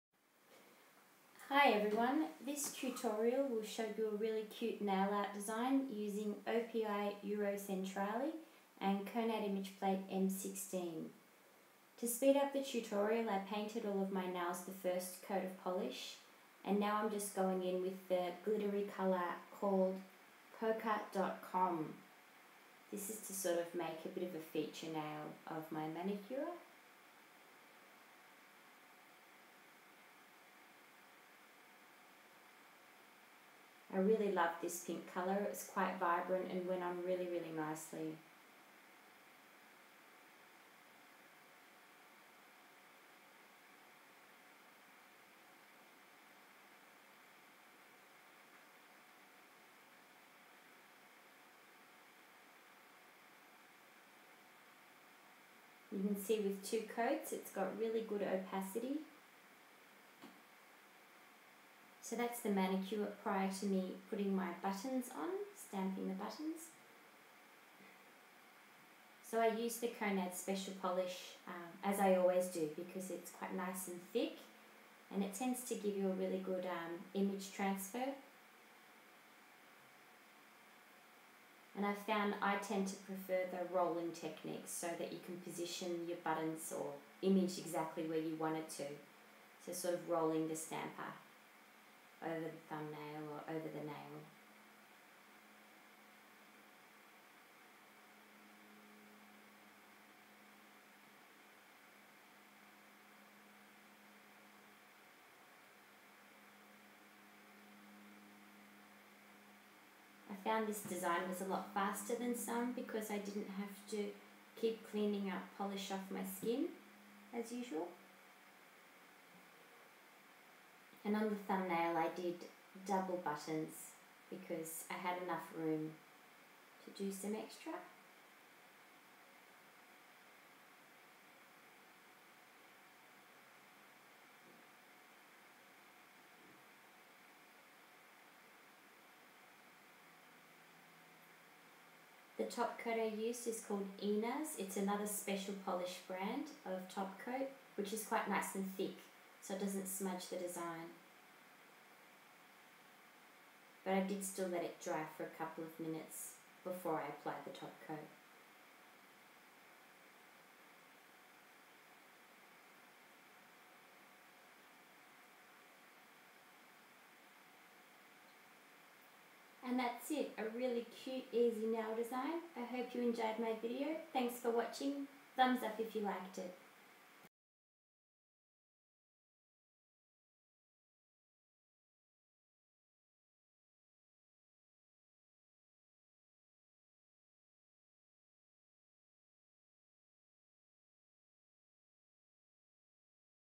Cute Button Nail Art
OPI Nail envy normal
OPI Euro centrale
Konad image plate M16
Konad special polish white
Enas top coat
Konad Scraper and stamper